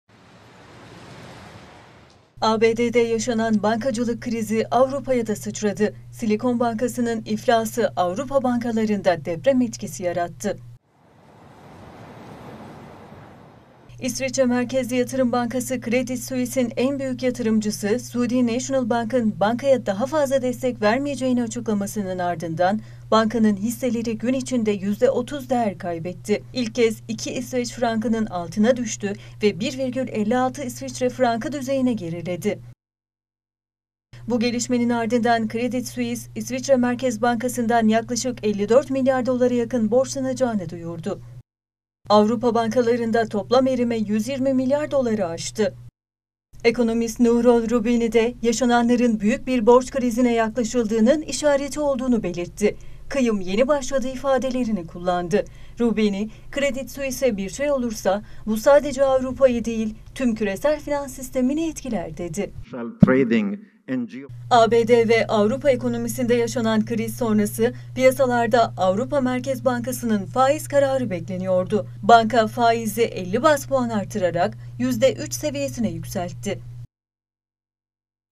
ABD'deki banka krizi Avrupa'ya sıçradı
ABD’de bankacılık sektöründe yaşanan kriz Avrupa’ya da sıçradı. İsviçre merkezli yatırım bankası Credit Suisse hisseleri yüzde 30 düştükten sonra İsviçre Merkez Bankası’ndan destek istedi.